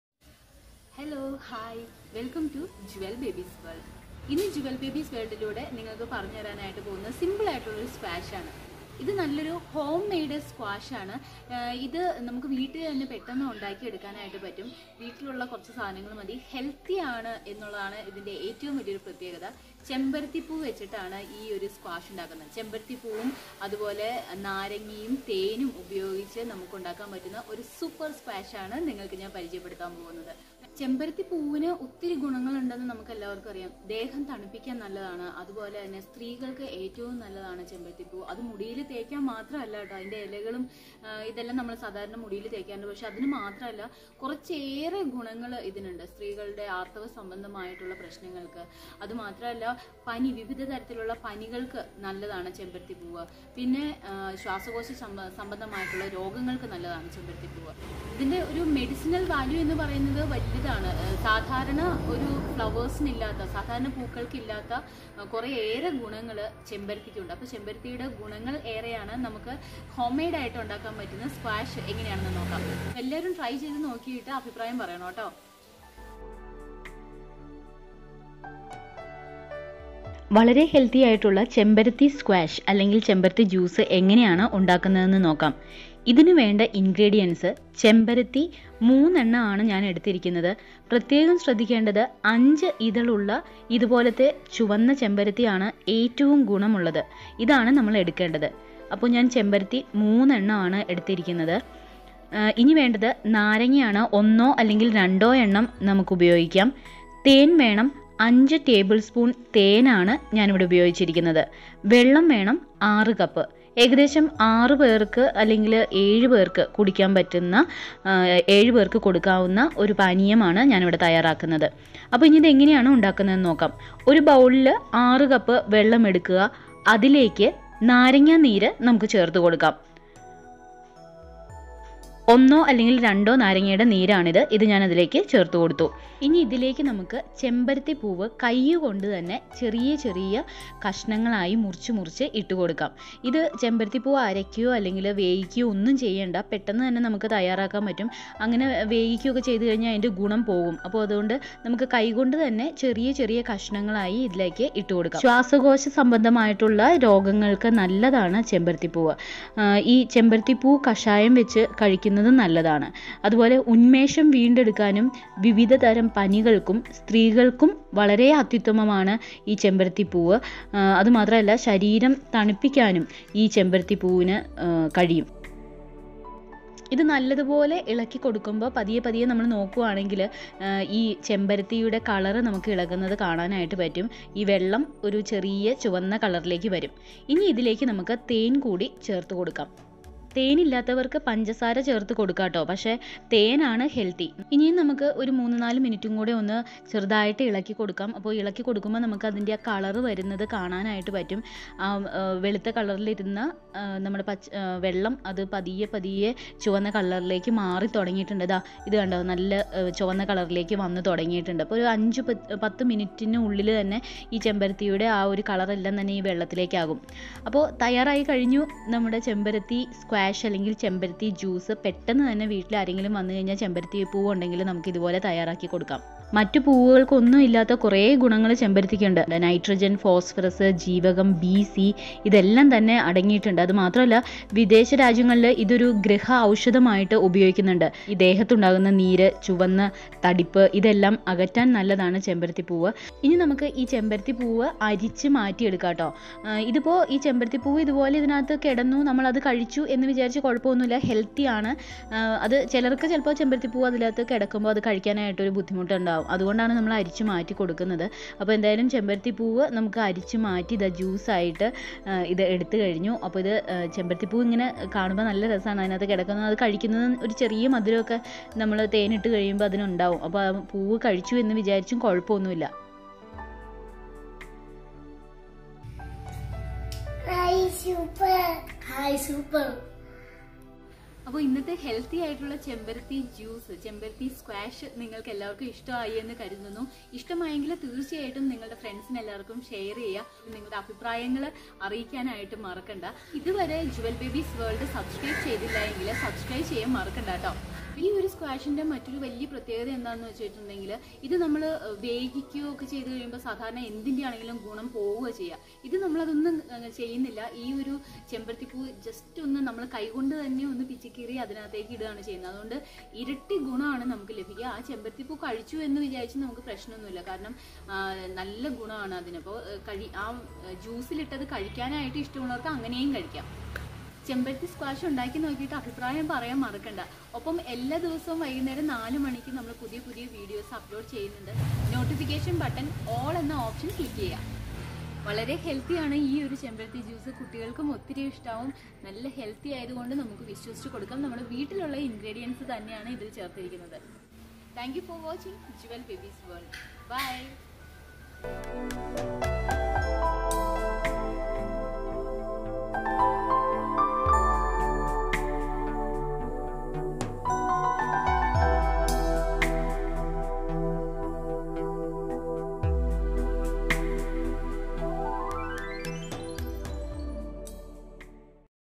🌺 ചെമ്പരത്തിപ്പൂവ് സ്ക്വാഷ് ശരീരം തണുക്കും രോഗങ്ങൾ പമ്പ കടക്കും | Hibiscus/Shoe Flower Healthy Drink
🌺ചെമ്പരത്തിപ്പൂവ് സ്ക്വാഷ് ശരീരം തണുക്കും രോഗങ്ങൾ പമ്പ കടക്കും|Hibiscus/Shoe Flower Sqush| Healthy DrinkChemparathi poovu🌺

ingredients:- shoe flower -3
                         Lemon-2
                          Water-6Cups
                           Honey-6 Tablespoon

This video takes you through the process of preparing Hibiscus Lemon Juice Recipe. Homemade juice with no preservative. try this...
Other Recipes .

●     ചക്ക ചവിണി കളയല്ലേ ?? ഉഗ്രൻ വട ഉണ്ടാക്കാം 🥓🥓🥓
ഉള്ളിവട മാറിനിൽക്കും !